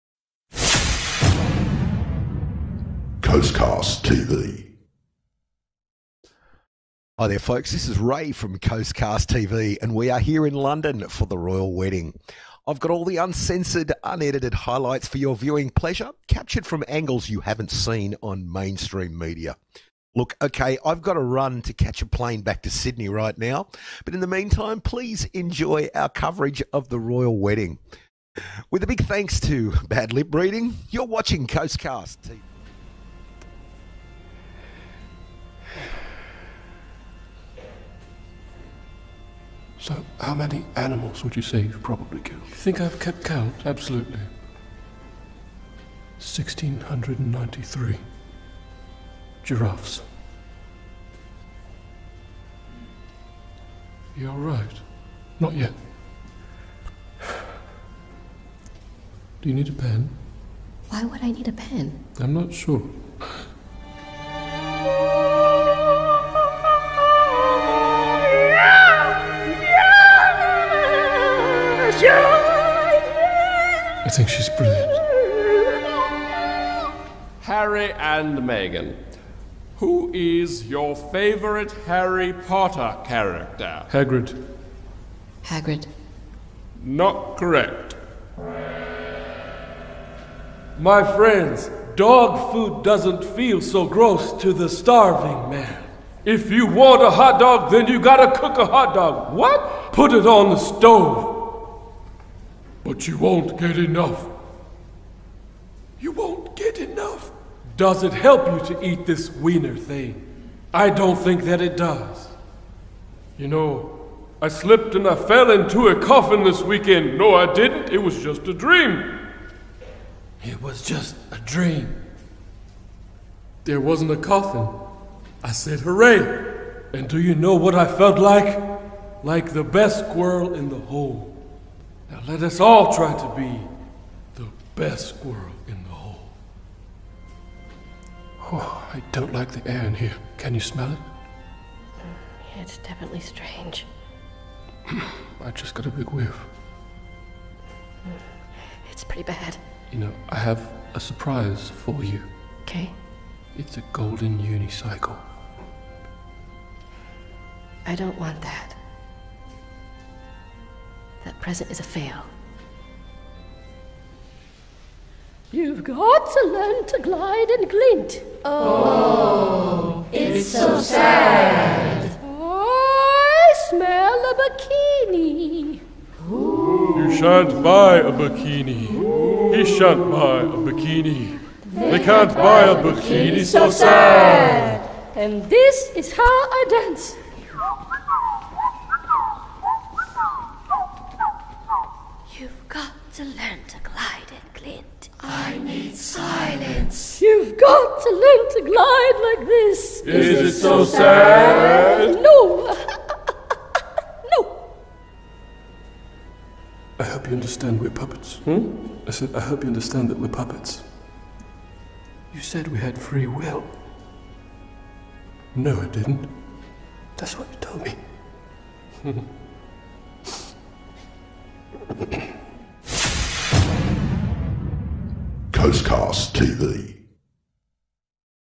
Royal Wedding CoastCast Highlights
With thanks to Bad Lip Reading, this is our coverage of the Royal Wedding in London